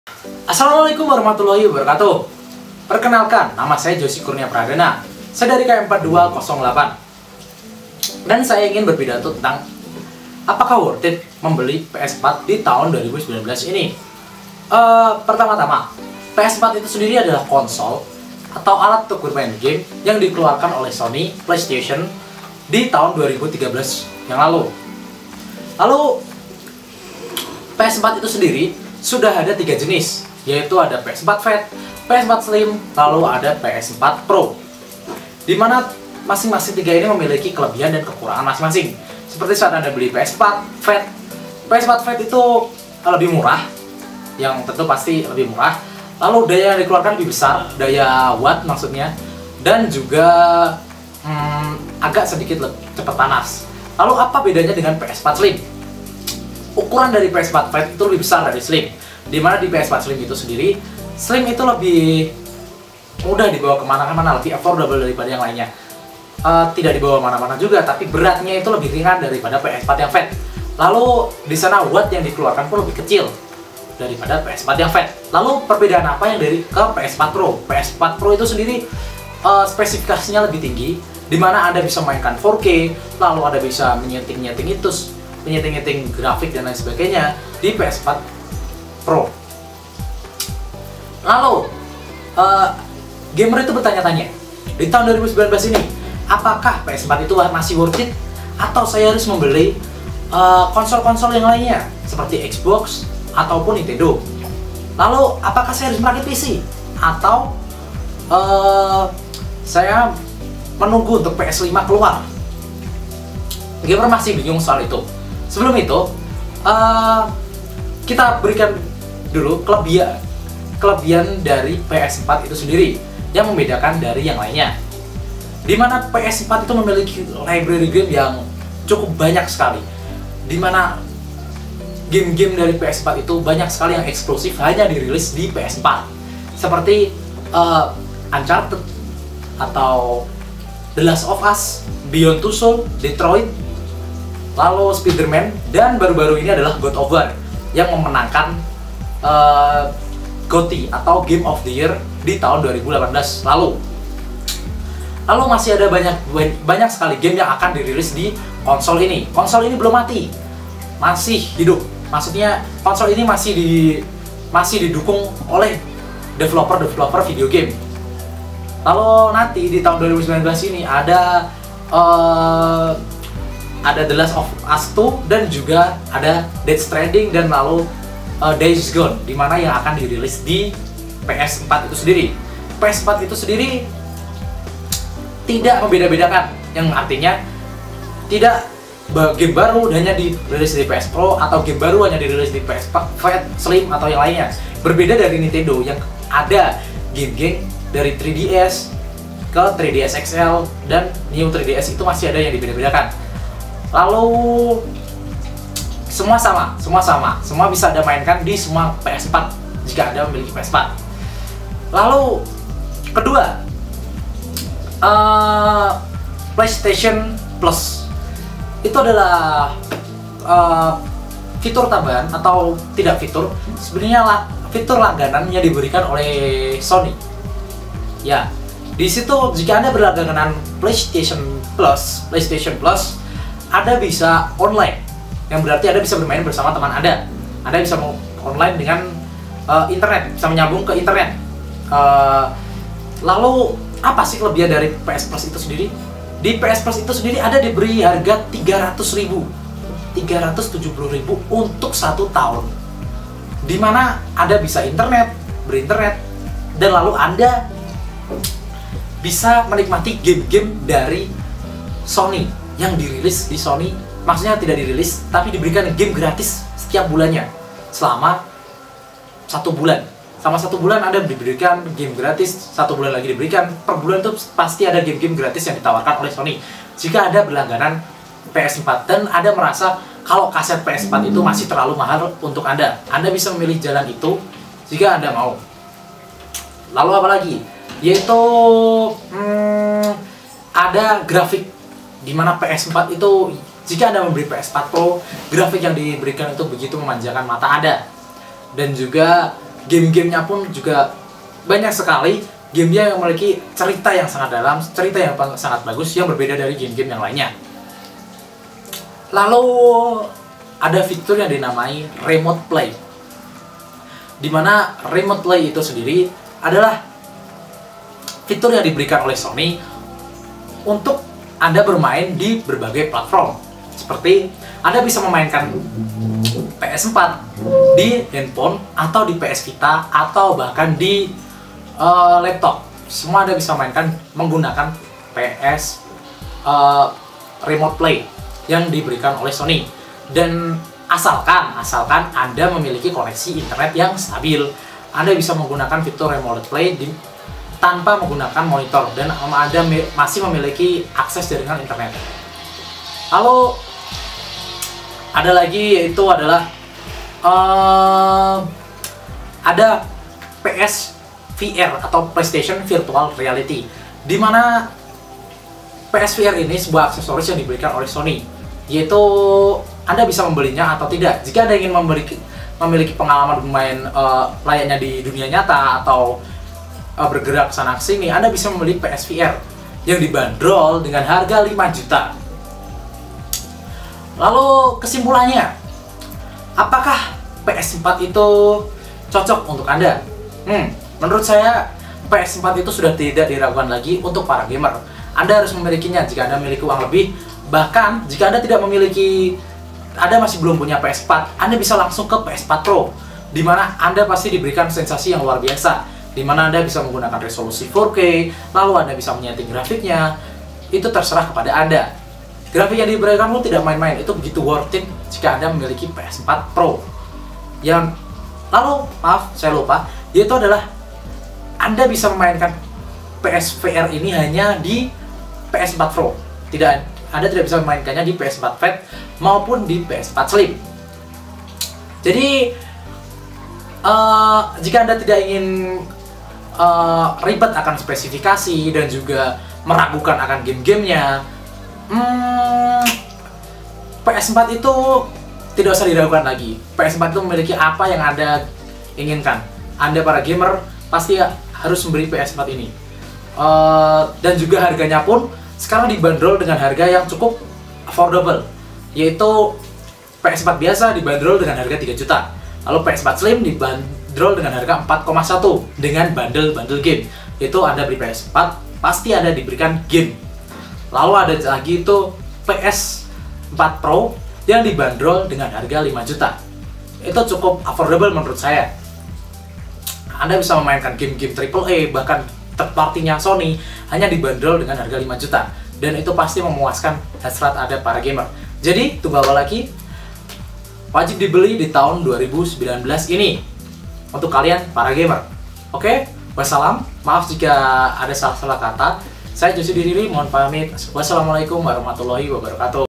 Alasan kenapa ps 4 layak sekali dibeli di tahun 2019Jhozy Kurnia p,KM 42 08
Nama :Jhozy Kurnia Pradana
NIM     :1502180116
Kelas   :Km 42 08

PlayStation 4 atau PS4 adalah konsol permainan video buatan Sony Computer Entertainment. Konsol yang diumumkan sebagai penerus PlayStation 3 dalam sebuah konferensi pers pada tanggal 20 Februari 2013 ini dirilis di Amerika Utara pada tanggal 15 November 2013, di Eropa dan Australia pada tanggal 29 November 2013, serta di Indonesia pada tanggal 9 Februari 2014, lebih dulu daripada tanggal rilis di Jepang pada 22 Februari 2014.[10][11] Di pasaran, PlayStation 4 berkompetisi dengan Nintendo Wii U dan Microsoft Xbox One sebagai salah satu konsol permainan video generasi kedelapan.

PlayStation 4 dibekali oleh prosesor APU x86-64 AMD dengan harapan agar dapat menarik minat lebih banyak pengembang dan dukungan bagi konsol tersebut. Prosesor grafis konsol ini mampu memproses 1,843 teraflop, tercepat di generasi ini.[12]

Sony tampaknya ingin memberi lebih banyak fokus terhadap cara bermain yang lebih sosial dengan menambahkan tombol berbagi di kontroler terbarunya untuk PlayStation 4 dan memungkinkan pemain menonton permainan video yang dimainkan teman secara langsung. Pada tahun fiskal 2014, permintaan atas konsol ini membantu Sony untuk memuncaki daftar penjualan konsol permainan video secara global.